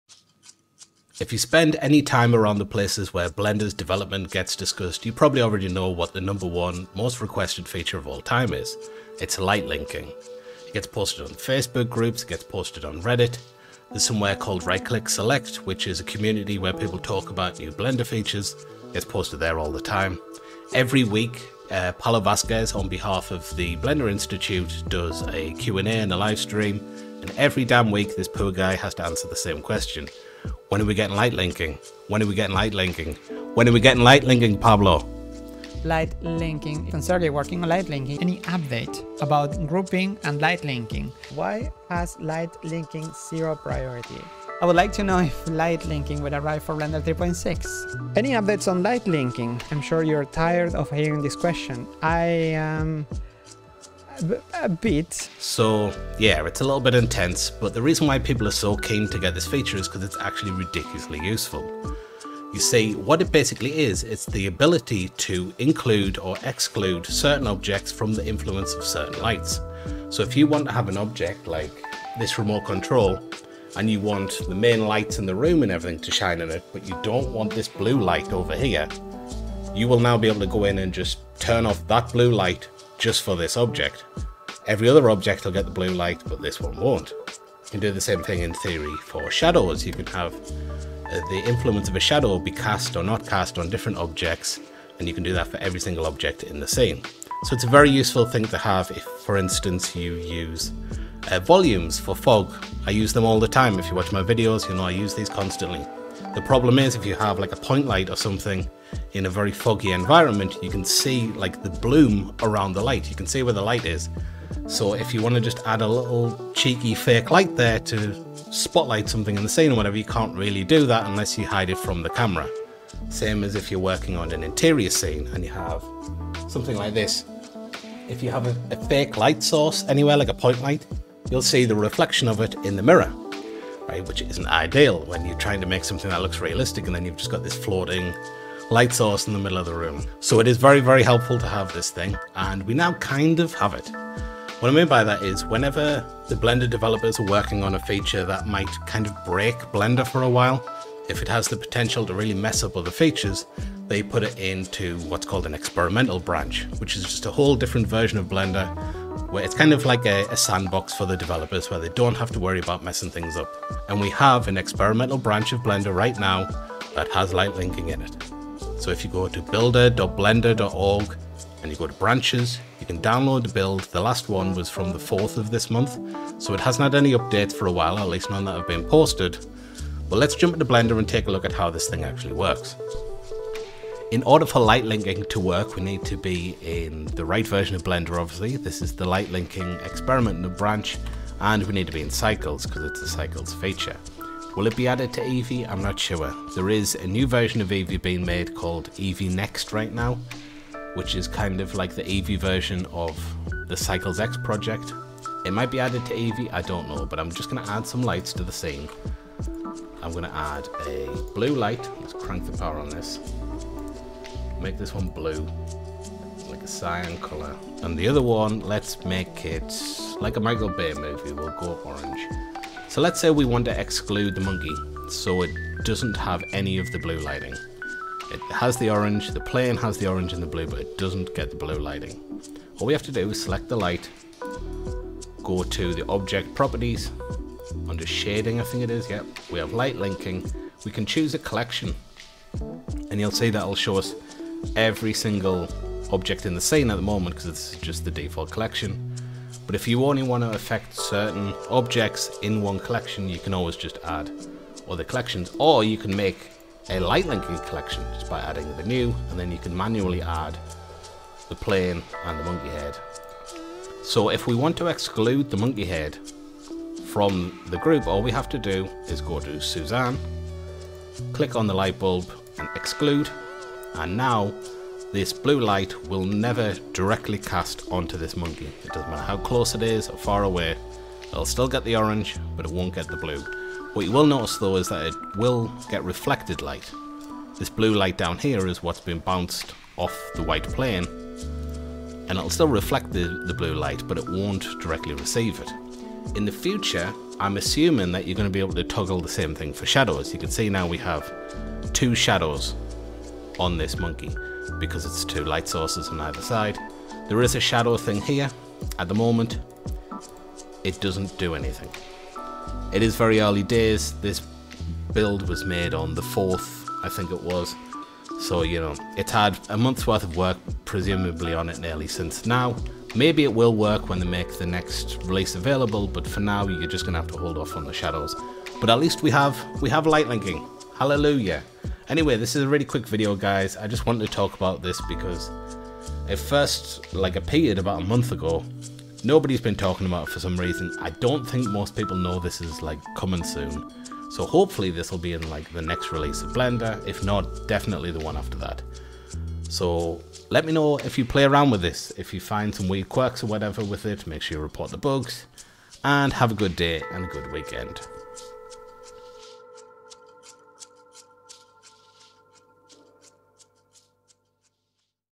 Blender's Most Requested Feature Is FINALLY HERE!
Yes, it's true. Lighting linking is finally coming to Blender!